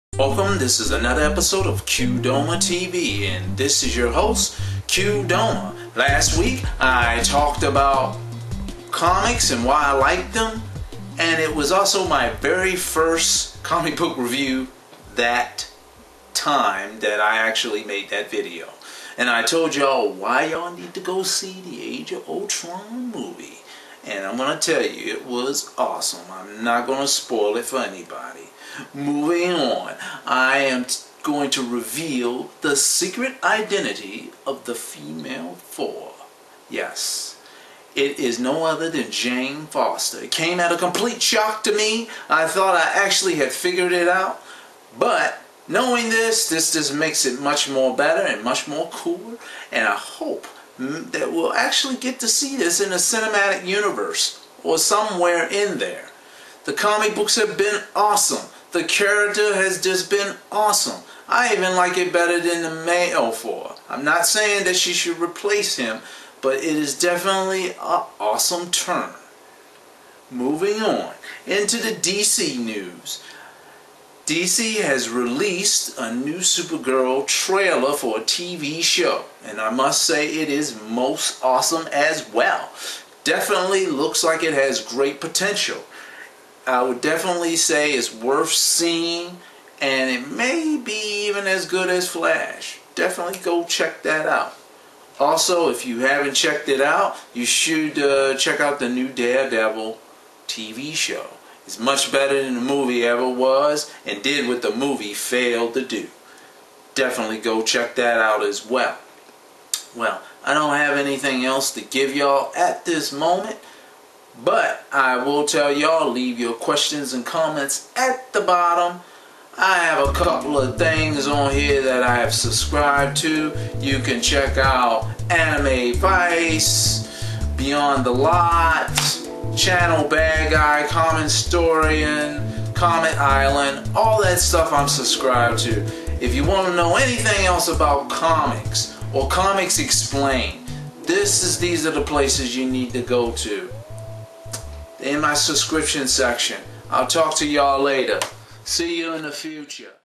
Talking about the new Super girl show and Identity of the female thor..
Doing my rant on the  Supergirl Tv shows  Thor and other things.